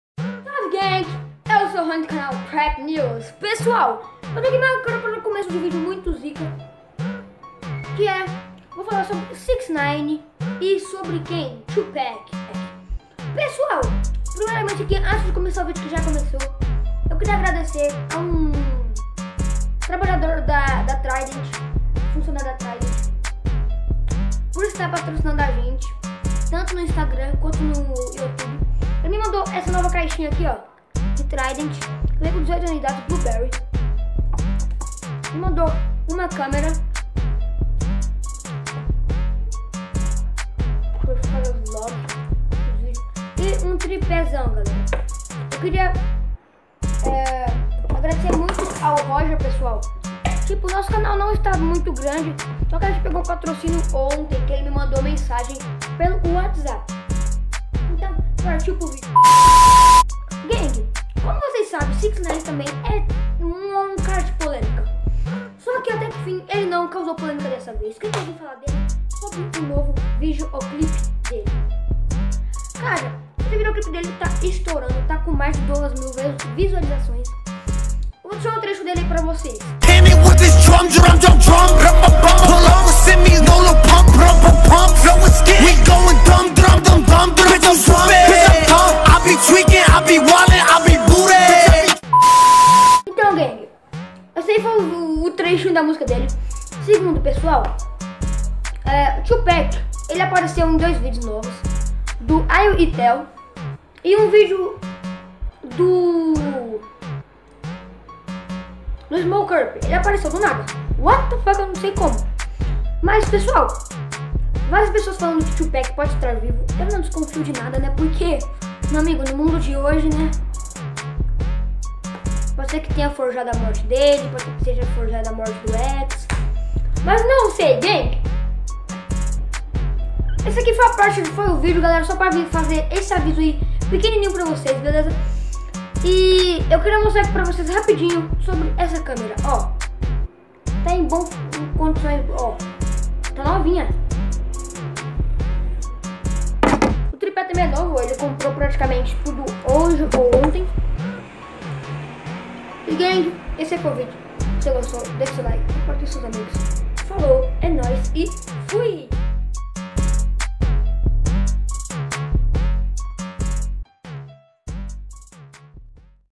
Agradecimento À Roger Pela Câmera E Pelo Tripé, Falando De: 6ix9ine E Tupac Está Vivo? (Trap News)
Obrigado Roger Pela Câmera E Pelo Tripé  Tamo Junto, Obrigado Mesmo.

E Aí Gang Eu Sou O Roni Do Canal Trap News E Nesse Vídeo Eu Falo Um Pouco De 6ix9ine E O Seu Novo Clipe, Falo Também Sobre As Teorias De Tupac Estar Vivo (Será Que É Verdade?)